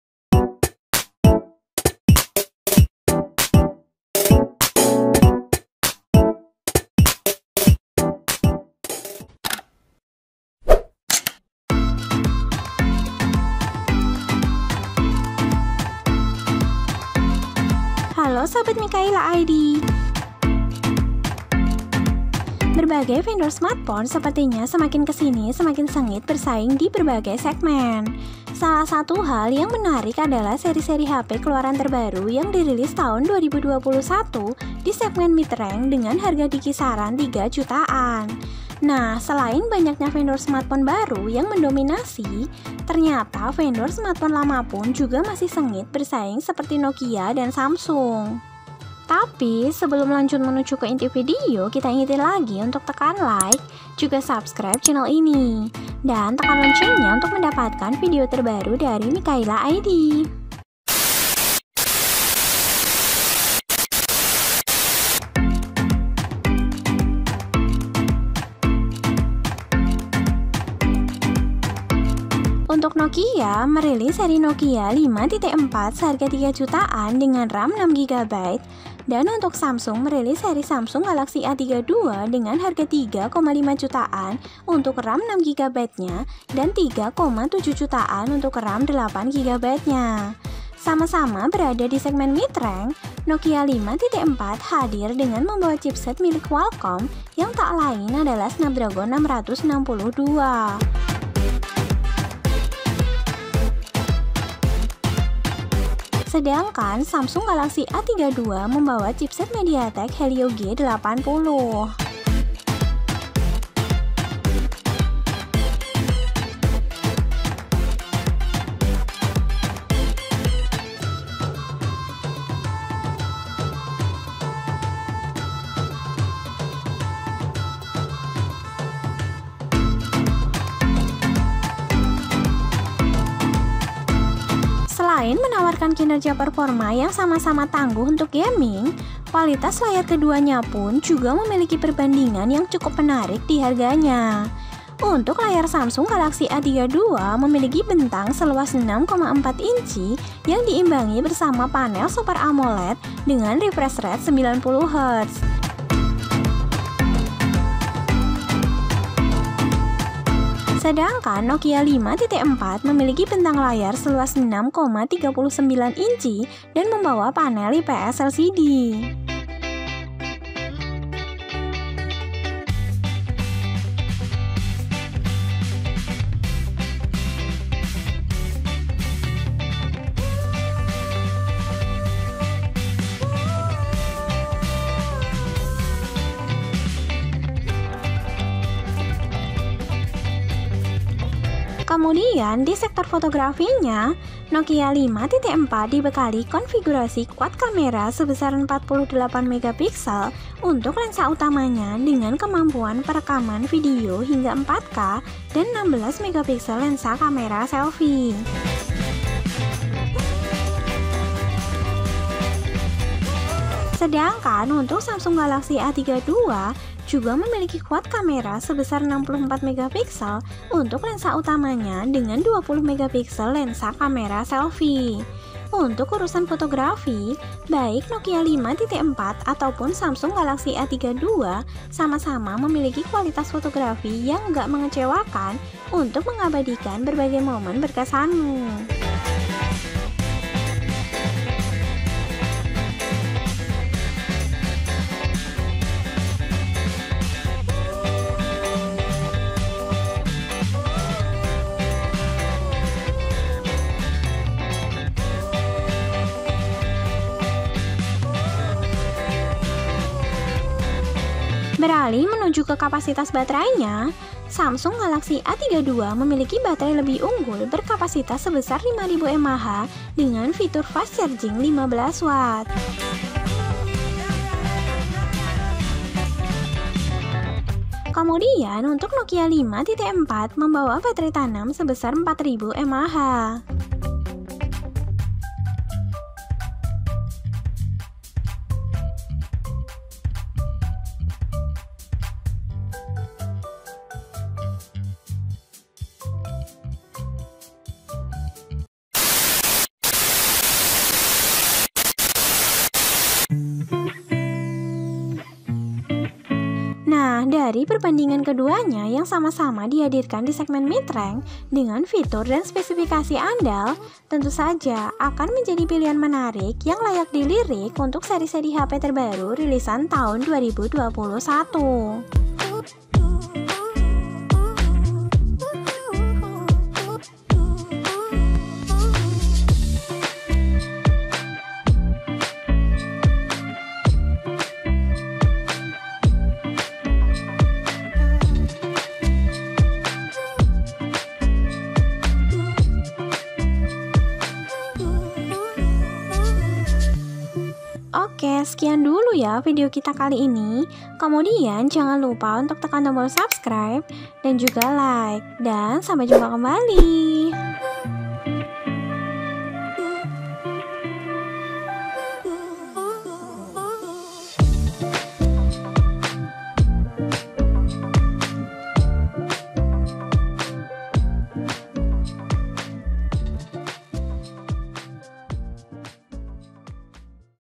Duel PANAS Pemain Lama!! HP 3 Jutaan Nokia VS Samsung, Kamu Pilih Mana?
Halo sahabat mikhayla id… Berbagai vendor smartphone sepertinya semakin kesini semakin sengit bersaing di berbagai segmen. Salah satu hal yang menarik adalah seri-seri hp keluaran terbaru yang dirilis di tahun 2021 di segmen mid-range dengan harga dikisaran 3 jutaan. Nah, selain banyaknya vendor smartphone baru yang mendominasi.. ternyata vendor smartphone lama pun juga masih sengit bersaing seperti Nokia dan Samsung.

Note: Harga bisa berubah sewaktu-waktu

Terimakasih